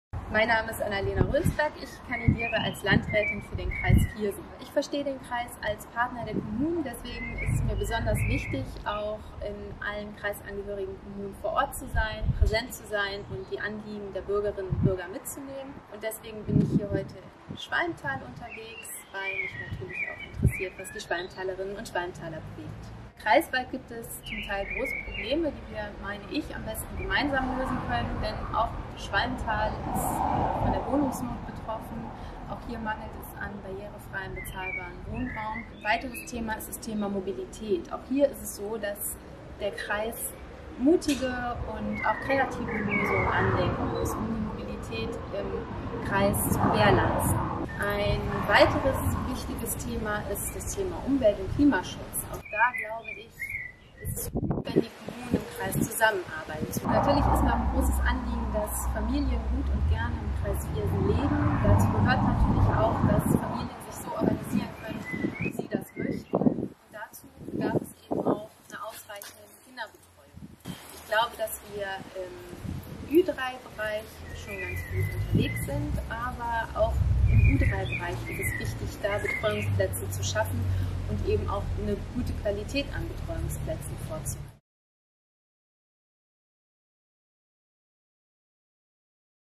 Annalena Rönsberg in Schwalmtal
Annalena Rönsberg ist Landratskandidatin der SPD für den Kreis Viersen. In diesem Video stellt Sie dar, in welchen Bereichen der Kreis stärker mit den Kommunen zusammenarbeiten kann.

Am 13. September in Kommunalwahl, auch in Schwalmtal. Bei der Landratswahl bittet die SPD Schwalmtal für Ihr Kreuz bei Annalena Rönsberg!